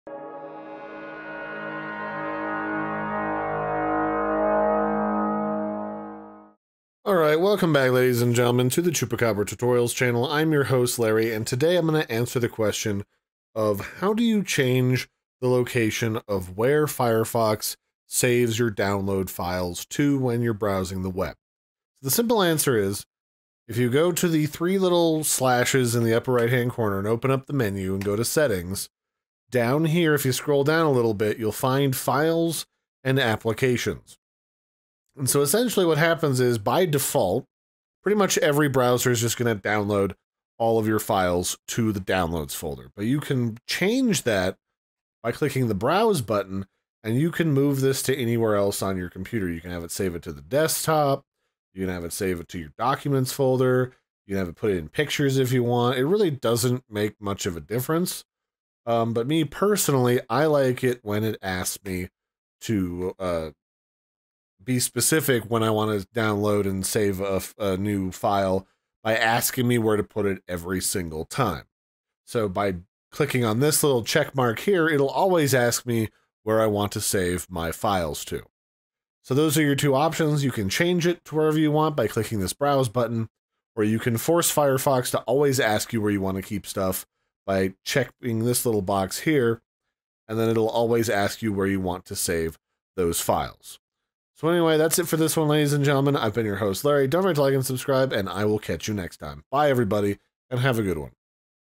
How to Change the Default Location of Firefox File Downloads - 2024 User Guide
How to change the default download location for files when using Firefox. Just head to your settings and look up "Download" in the settings search bar. Here you can change the default location to any file/folder location you want on your computer. You can also make firefox ask you every time you download a file where you want to save it. Updated for 2024.